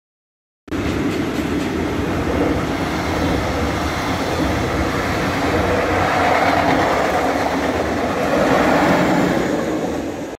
1144 226-8 am REX/R5908 in Linz Untergaumberg am 14.11.2023
REX/R 5908:
Linz Hbf 12:50
Marchtrenk: 13:01-13:02
Wels Hbf 13:07-13:09
Haiding: 13:14-13:14
Bad-Schallerbach Wallern: 13:18-13:19
Schlüßlberg: 13:23-13:23
Grieskirchen-Gallspach: 13:26-13:27
Obertrattnach-Markt Hofkirchen: 13:31-13:31
Neumarkt Kallham: 13:35-13:36
Kimpling: 13:40-13:40
Kumpfmühl: 13:44-13:44
Riedau: 13:47-13:48
Zell/Pram: 13:50-13:50
Andorf: 13:56-13:56
Taufkirchen/Pram: 14:00-14:01
Schärding: 14:08-14:09
Wernstein: 14:13-14:14
Passau Hbf: 14:22